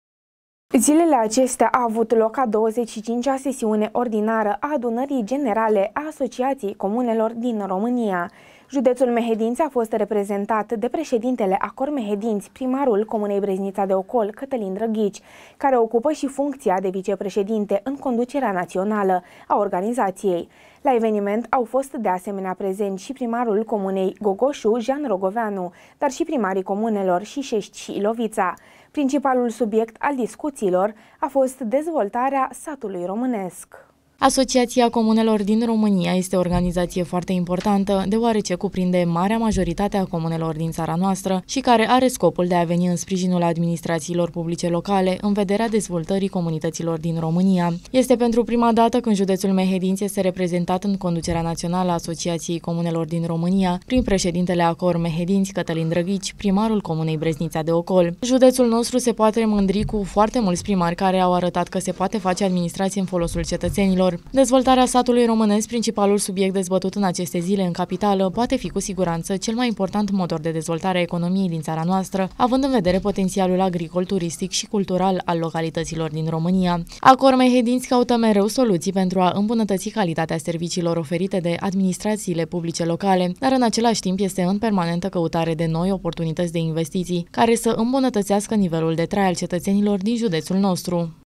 MEHEDINȚIUL REPREZENTAT ÎN CONDUCEREA NAȚIONALĂ A ASOCIAȚIEI COMUNELOR DIN ROMÂNIA
DEZVOLTAREA SATULUI ROMÂNESC - PRINCIPALUL SUBIECT DEZBĂTUT ZILELE ACESTEA LA BUCUREȘTI